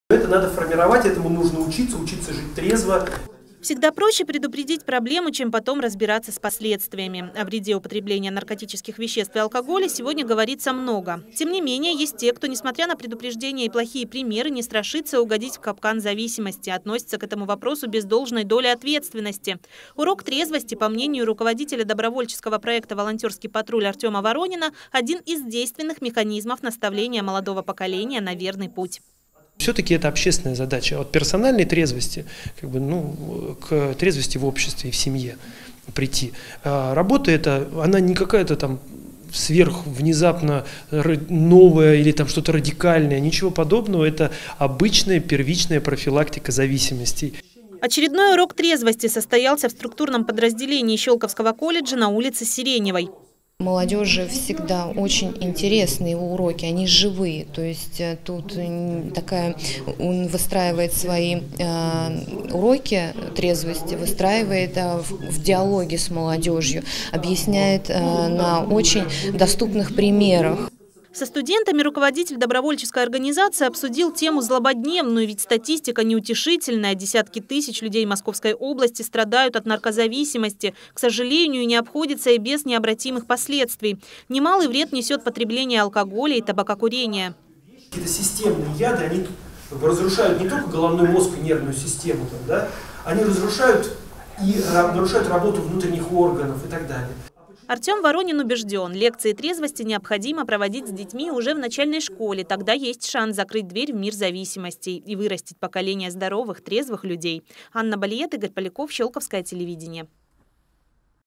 «Урок трезвости» состоялся Щёлковском в колледже
В Щёлковском колледже прошел «Урок трезвости». О вреде курения, употребления алкоголя и наркотиков со студентами поговорил руководитель добровольческой организации «Московский областной волонтерский патруль» Артем Воронин.